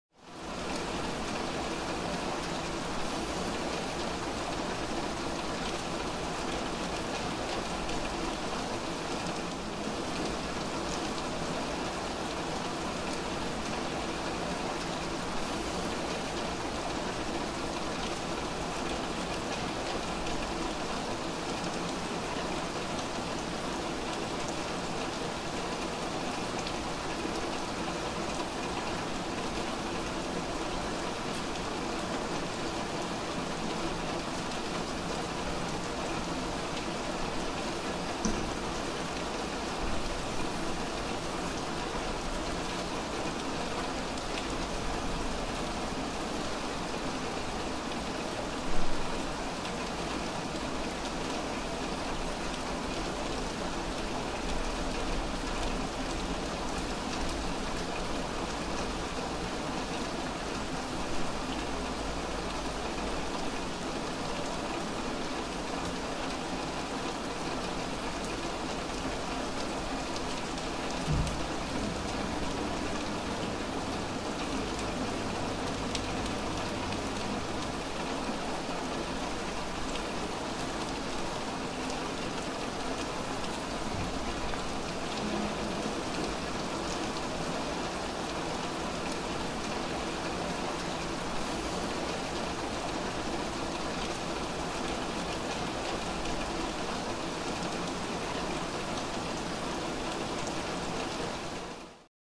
Lluvia.
Aún confinado no hay razón para estarse sin hacer nada. La lluvia es una buena razón para tomar unos planillos de recursos.